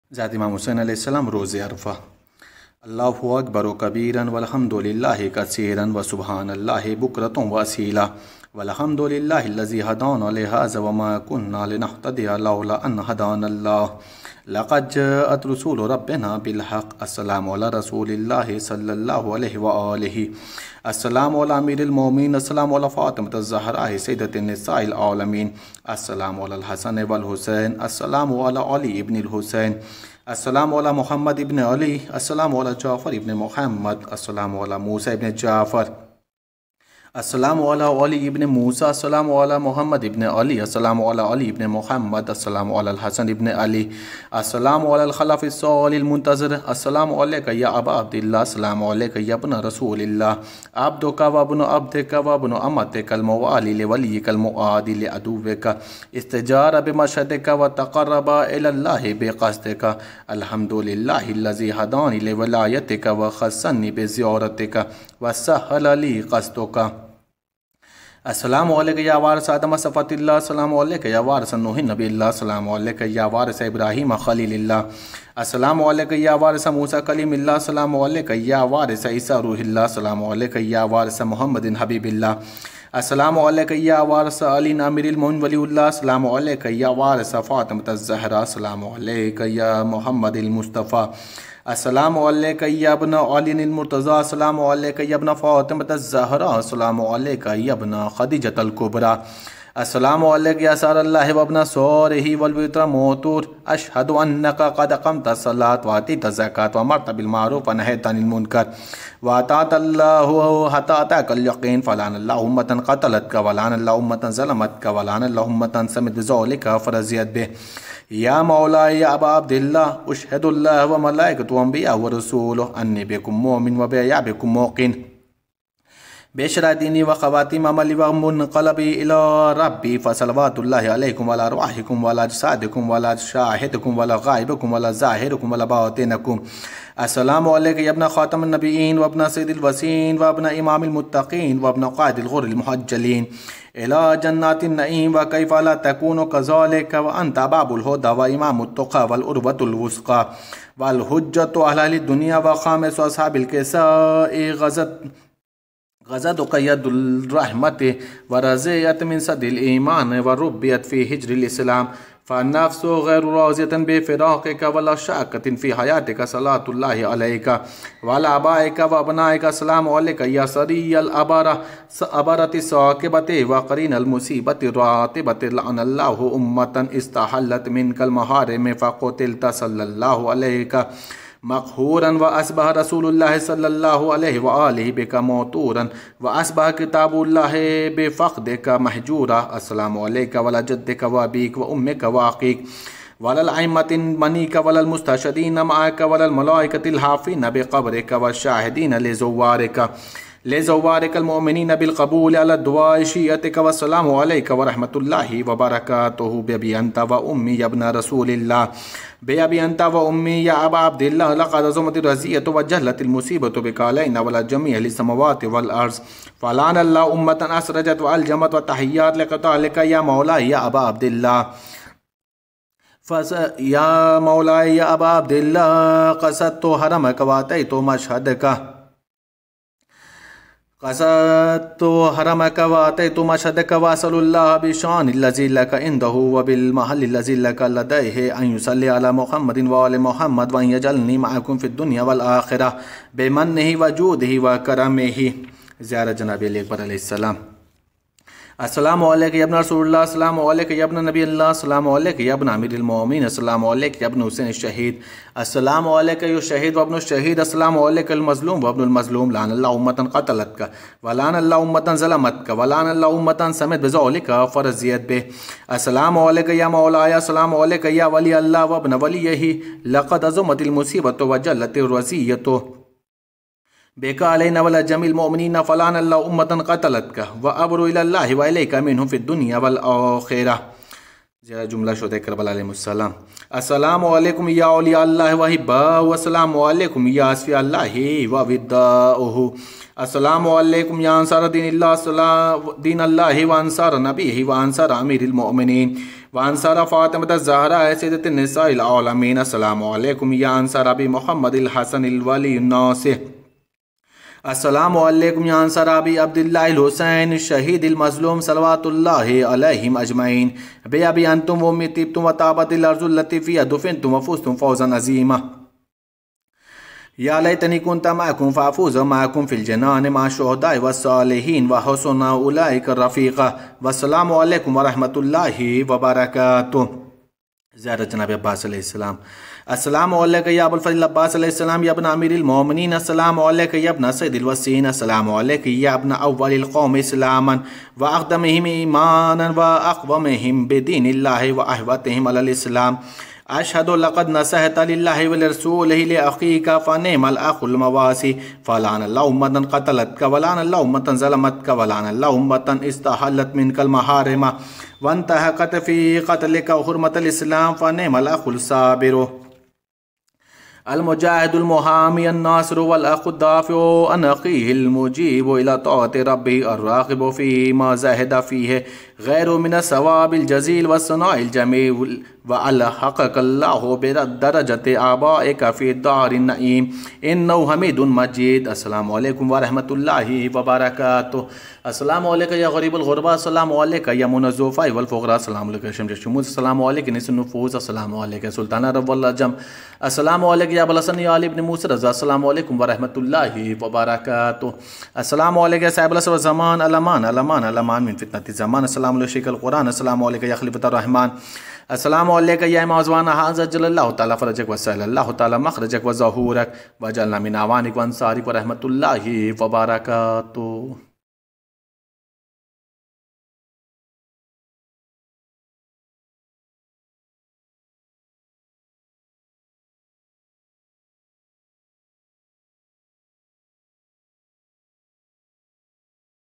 Ziyarat E Imam Hussain (a.s) specific on the day of Arafah | Ziyarat E Arafa | Karbala |2020/1441 HD
THIRD COMPREHENSIVE FORM OF ZIYARAH
‘Allamah al-Majlisi, in his book of Tuhfat al-Za’ir, has listed the following comprehensive form of ziyarah as the eighth, saying: This form of ziyarah has been reported by Sayyid Ibn Tawus within the supplicatory prayers of the ‘Arafah Day from Imam al-Sadiq, peace be upon him. It can be said at visiting any shrine at any time, especially on the ‘Arafah Day. It is as follows:

Recitor - Maulana Muttahir Hussain Rizvi

All contents of this channel are copyright protected.

©&® Nauha Tube